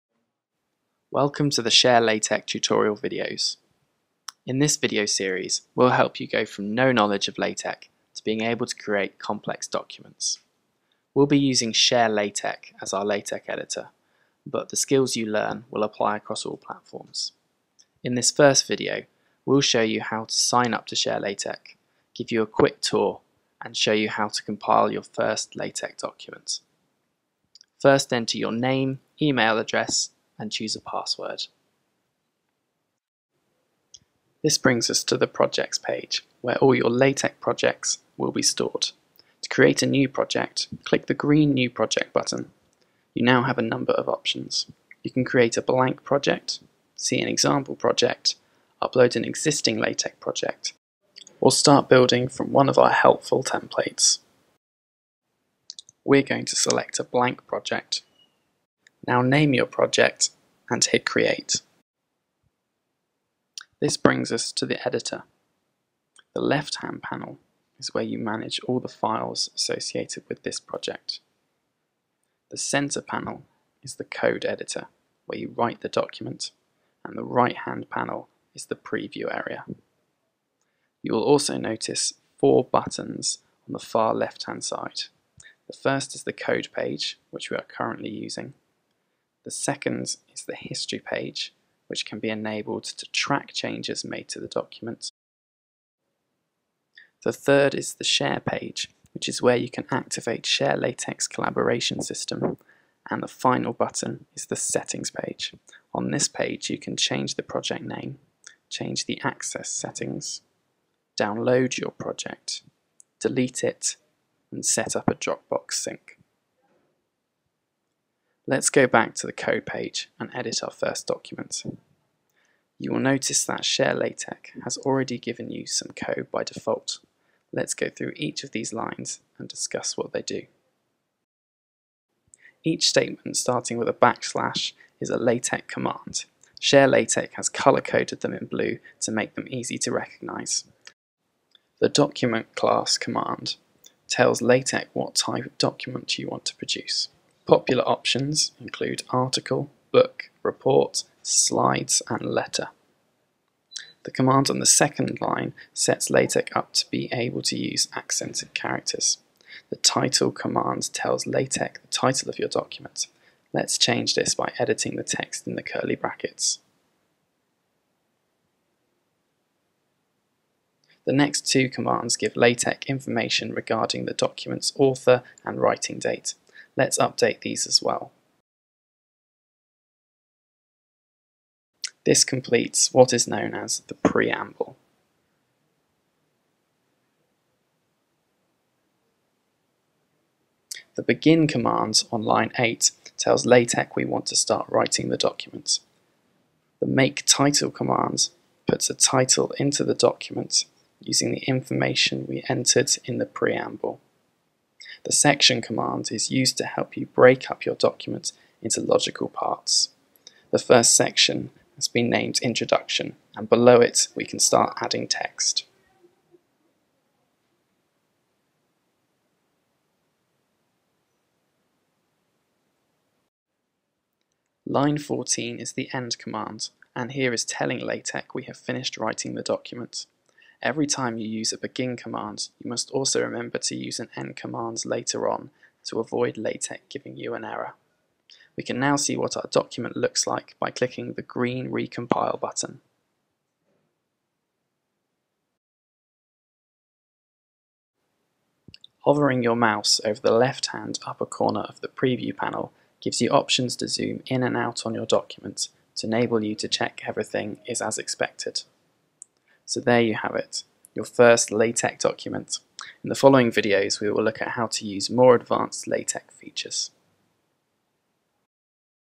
LaTeX Tutorial pt 1 - Creating Your First Document
In this video series we'll take you from no knowledge of LaTeX to being a competent user. We'll be using ShareLaTeX as our LaTeX editor but you can learn with whatever editor you like. In this first video we look at getting started with ShareLaTeX, show you some of it's features and help you create your first document.